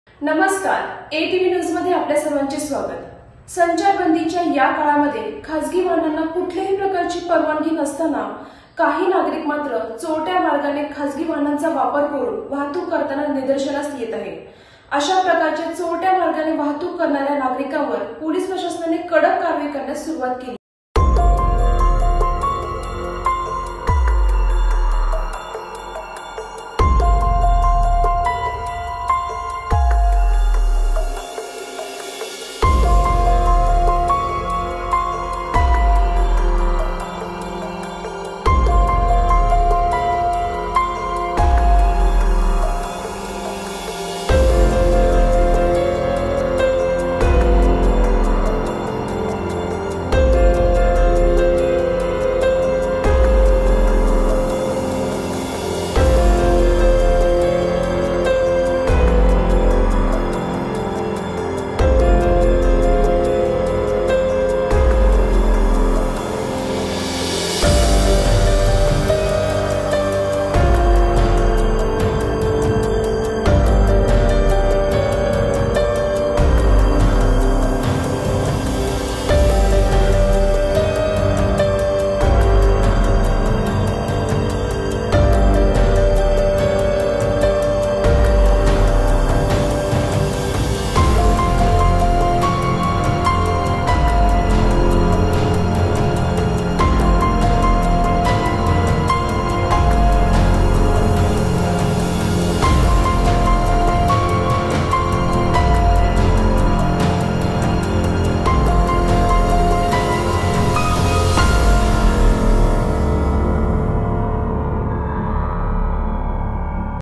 Atv news मुकुंदनगर भागात चोरट्या मार्गाने वाहतूक करणाऱ्यावर पोलिसांची कारवाई
Latest Ahmednagar City  NEWS update